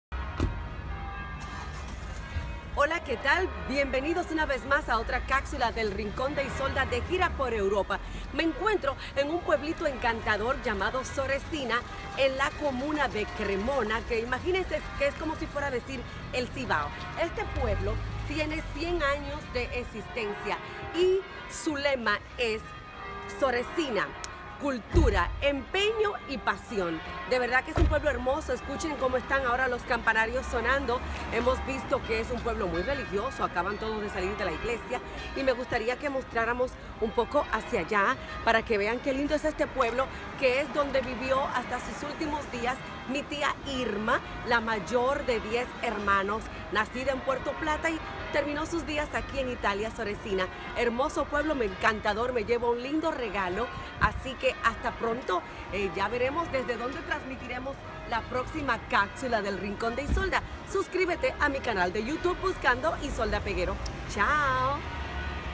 Soresina, Italia un pueblo con pasión
Un vistazo a Soresina al norte de Italia, un pueblo de diez mil habitantes en la region de Lombardía celebra su centenario.